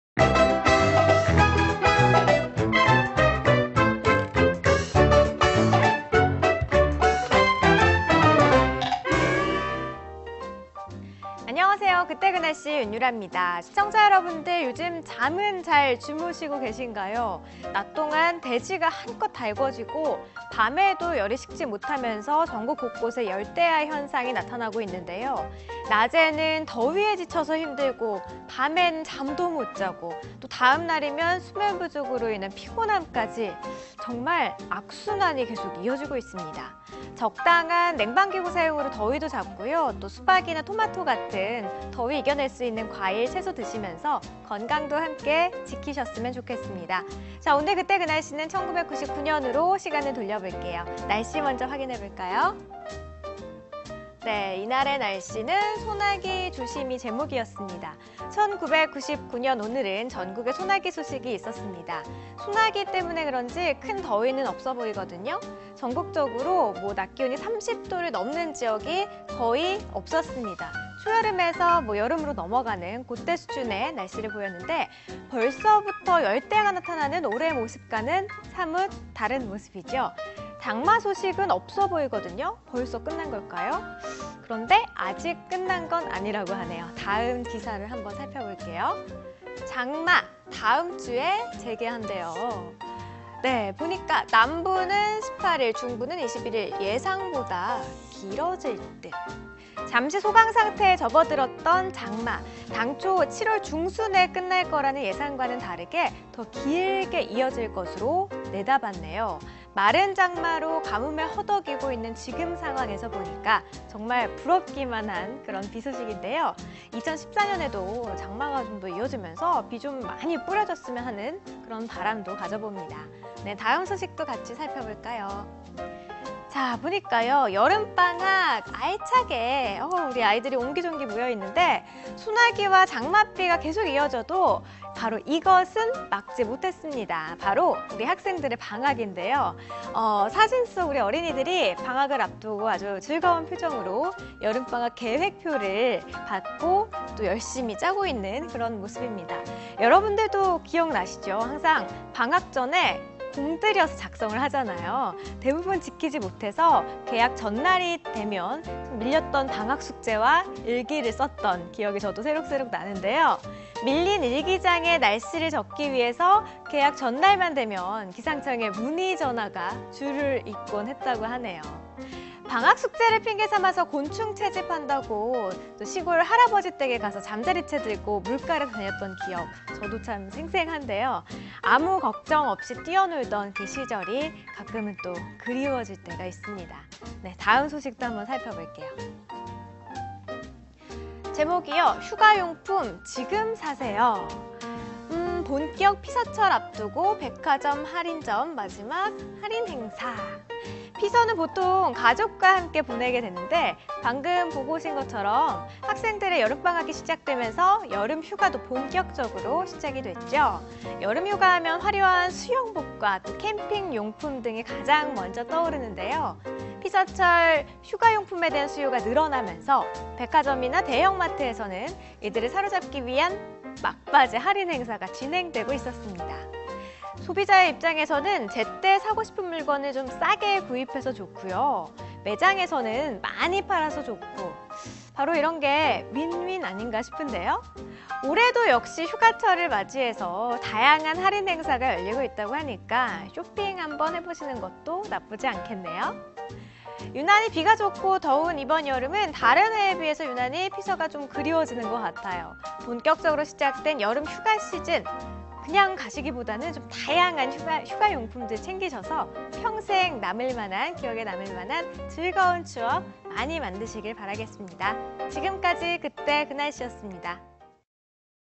그때 그 날씨 [07/16] / YTN 웨더
●소나기 조심- 온나라 구름끼고 소나기 오는 곳 있어●장마 다음주 재개- 남부 18일, 중부 21일 예상보다 길어질듯●여름방학 알차게- 서울 영등포구 여의도초등학교●휴가용품 지금 사세요- 본격 피서철 앞두고 백화점·할인점 막바지 할인